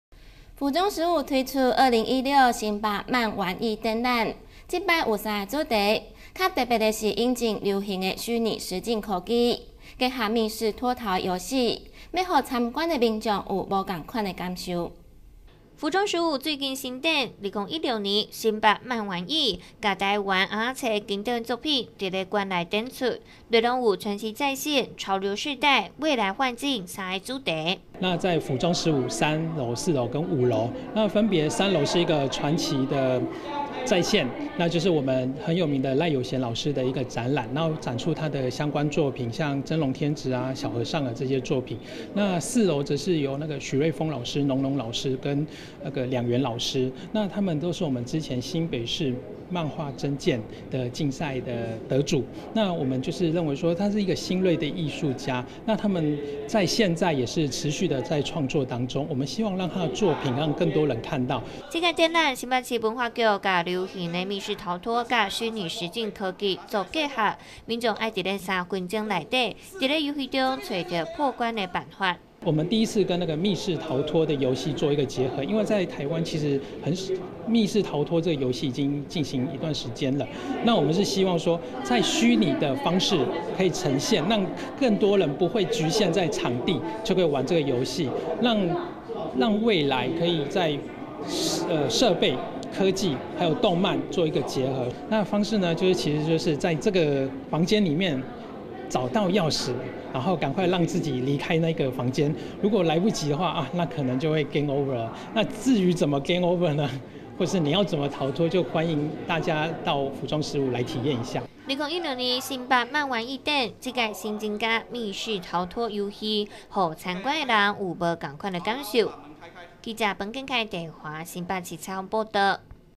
NTCnews 20161014 09台府中15新展 2016新北漫玩藝
府中15推出「2016新北漫玩藝」展，這次有「傳奇再現」、「潮流世代」、「未來幻境」三個主題，比較特別的是引進時下虛擬實境科技，結合密室逃脫遊戲，要讓參觀的民眾有不一樣的感受。